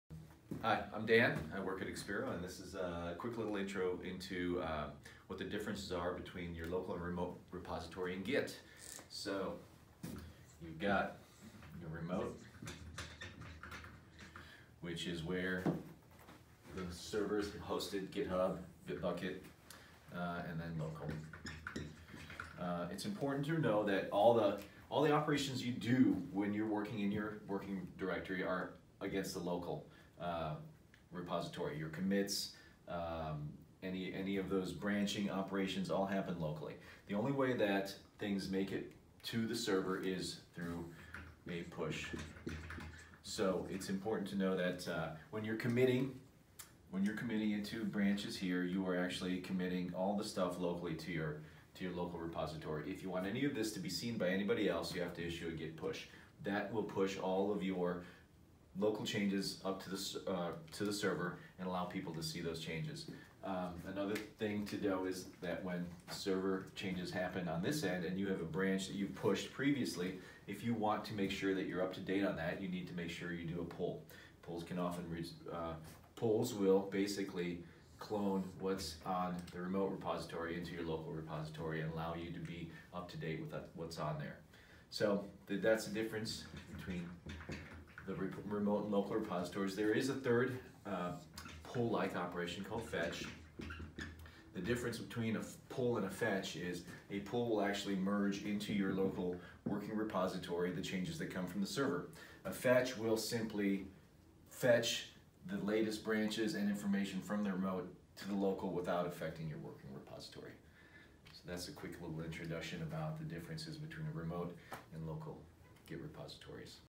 Local vs. Remote Repositories in Git
• Git Pushes
• Repository Cloning
• Git Fetching
• Git Pulls